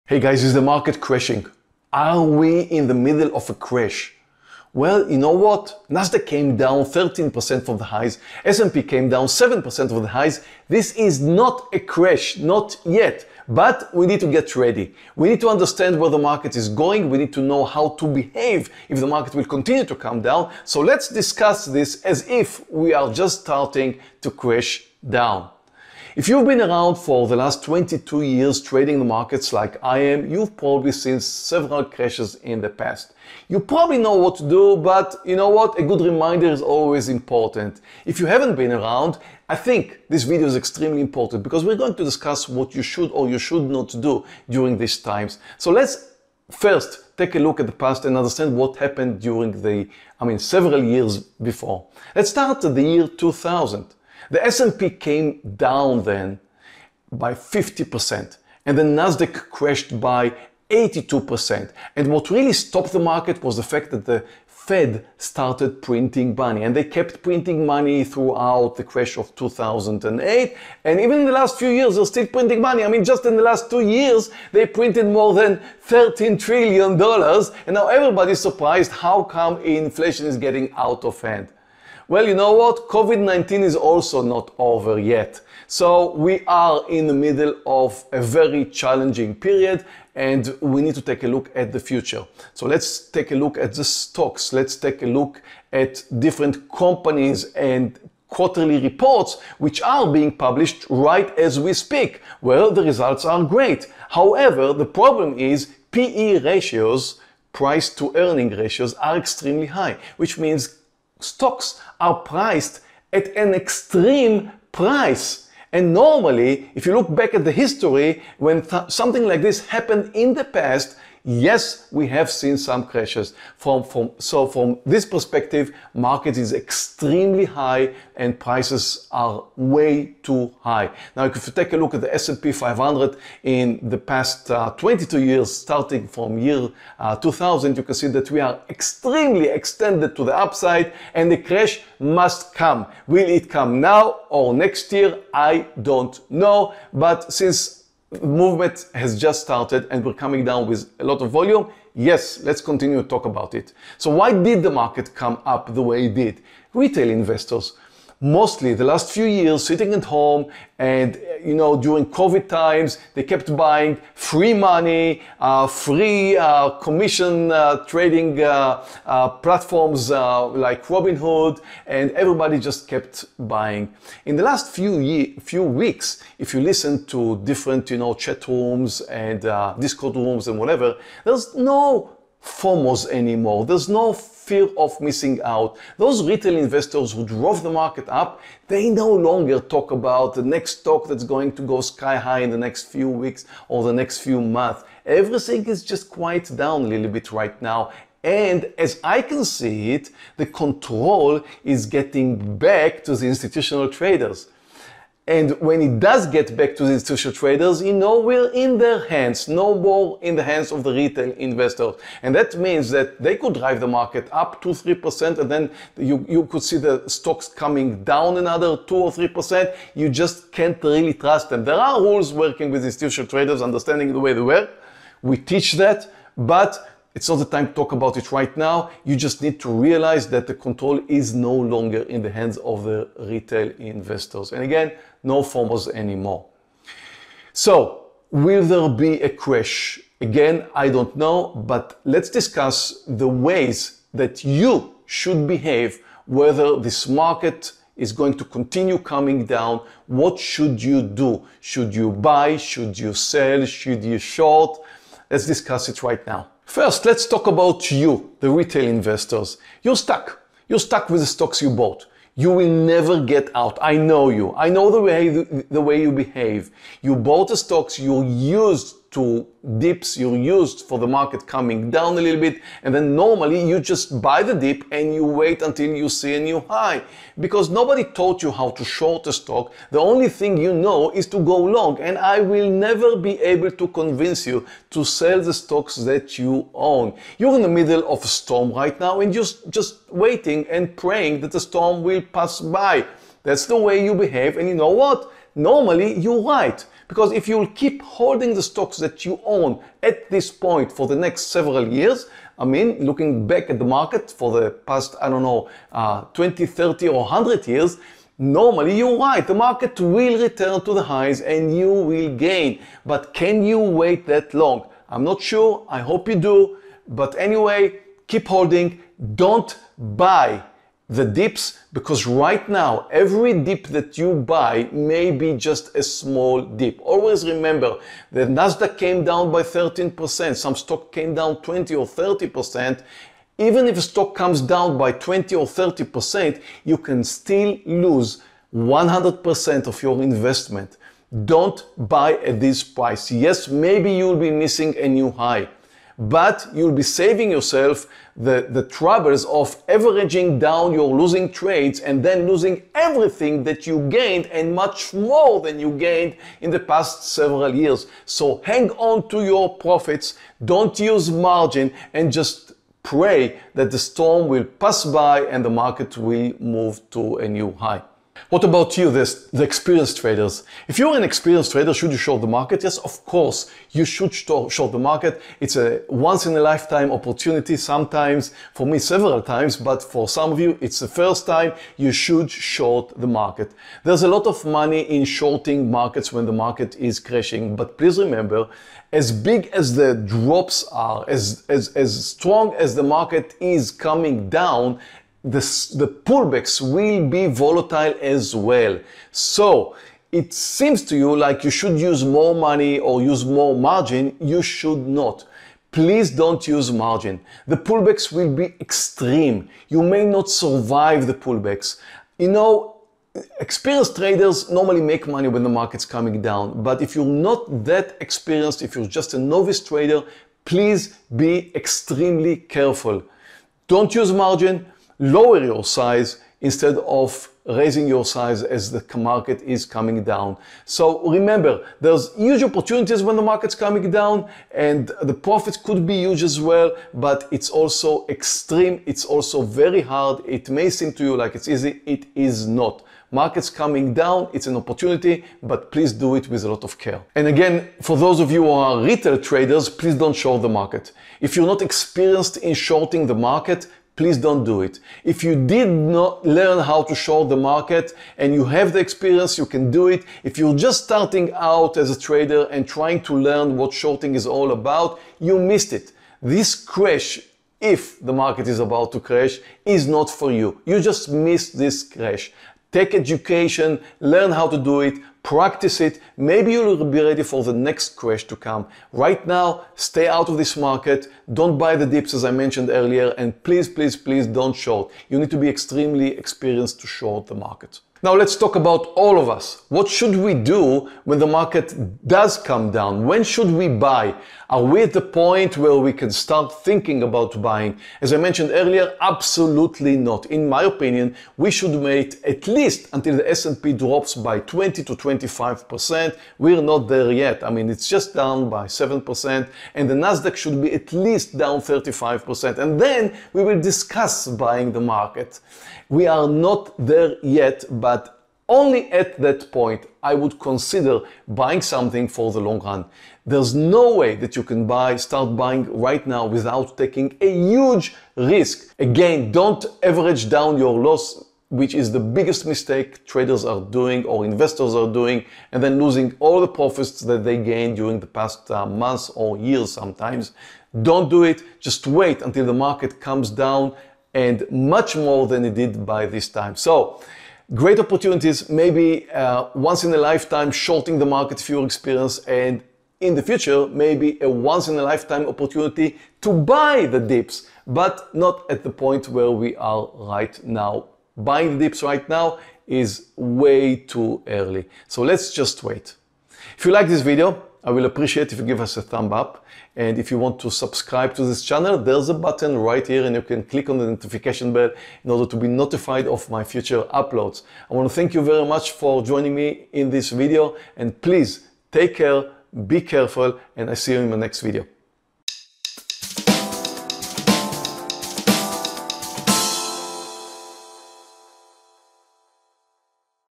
WILL THE MARKET CRASH IN 2022?? MY PREDICTION AFTER 22 YEARS OF TRADING EXPERIENCE…
Will the stock market crash in 2022? What should you do? What shouldn't you do? How do you prepare? Here's my prediction after 22 years of trading experience...